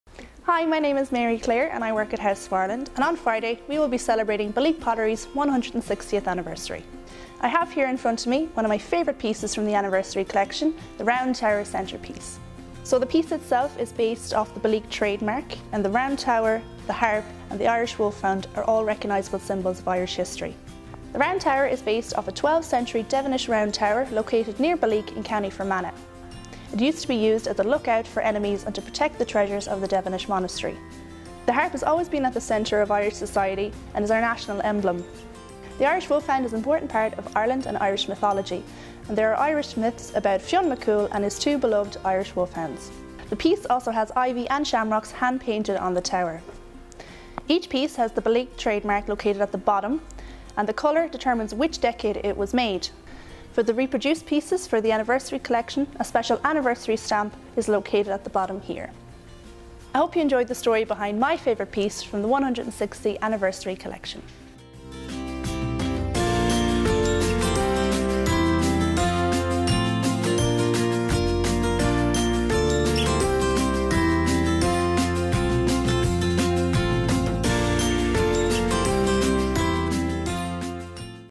The Story of the Round Tower Centrepiece by Belleek
We are celebrating the 160th Anniversary of Belleek Pottery, this Friday the 9th of June, in all our House of Ireland stores! Mary-Claire, one of our sales advisors, has chosen her favourite piece, the Round Tower Centerpiece, from the Belleek's 160th anniversary collection and here, tells its story.